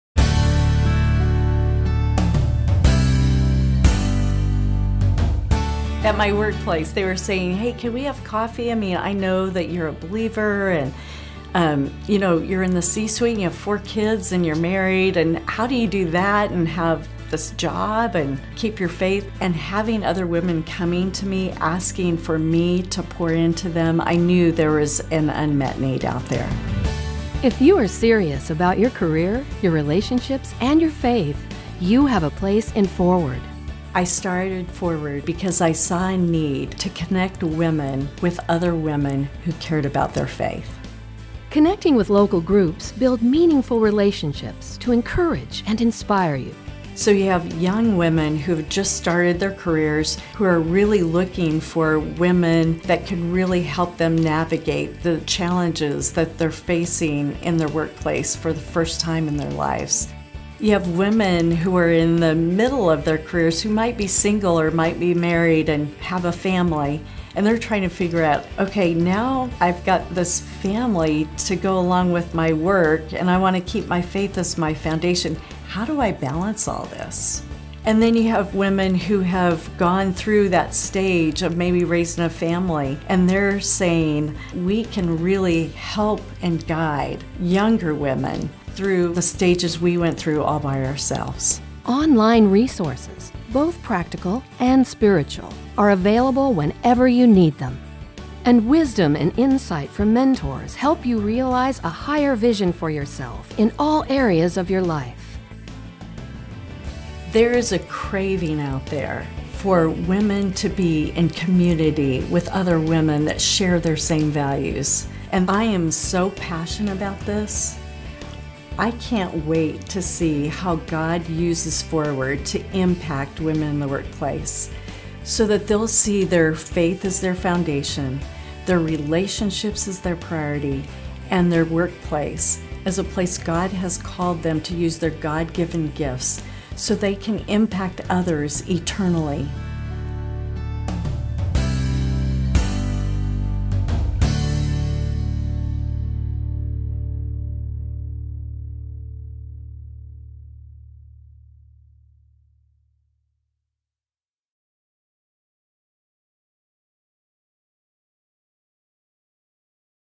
About 4word 2016
4word Founder, Diane Paddison, shares the mission, vision, and programs for 4word including the events that helped her realize why connecting Christian women in the workplace is so important and needed.